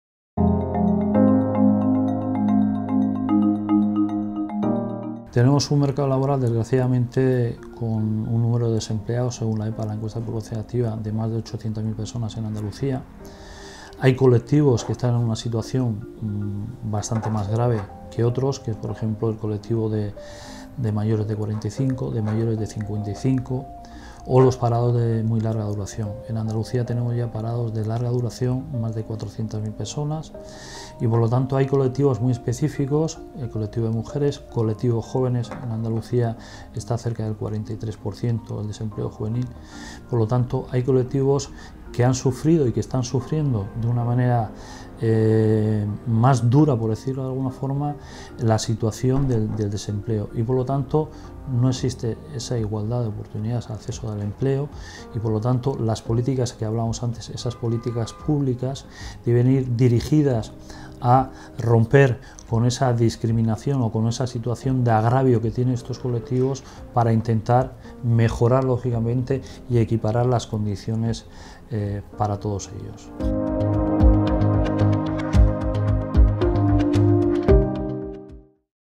¿Hay Igualdad de condiciones en el ámbito laboral para todos los colectivos? Oskar Martín
DESDE DDF INICIAMOS UN PROYECTO DE SENSIBILIZACIÓN E INTERVENCIÓN BAJO EL MENSAJE “EL TRABAJO DIGNO ES UN DERECHO DE TODOS”.

OSKAR MARTÍN, SECRETARIO DE FORMACIÓN Y EMPLEO DE UGT-A NOS RESPONDE A UNA SERIE DE PREGUNTAS SOBRE EL PANORAMA LABORAL ACTUAL Y SUS DESAVENENCIAS, REFLEJANDO LA VERDADERA REALIDAD DE TEMAS COMO LA CONCILIACIÓN LABORAL, EL TRABAJO DIGNO O LA IGUALDAD EN EL PUESTO DE TRABAJO.

¡¡¡ ÚNETE A NUESTRO COMPROMISO, COMPARTE TU HISTORIA Y DIFUNDE!!!